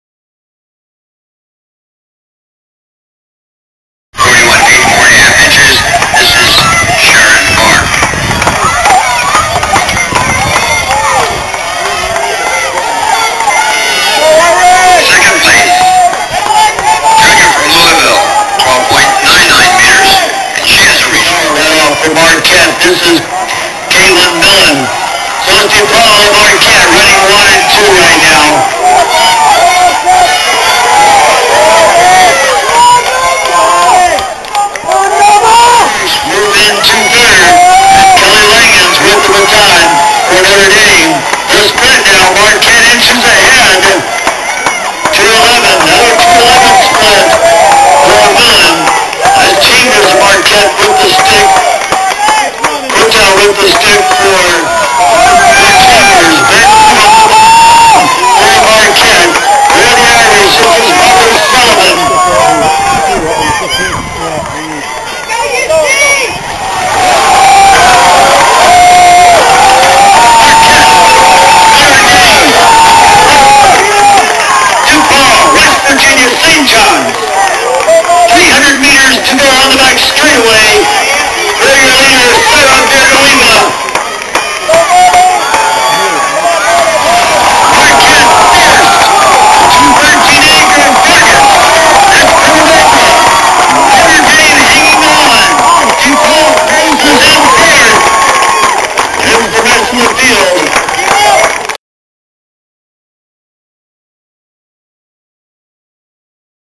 DePaul Women's 4x800m Relay at 2008 BIG EAST Championships
Claire Duck, Allyson Petersen, Maureen Kuhta and Mary Cate Quiett set a DePaul record at the 2008 BIG EAST Championships.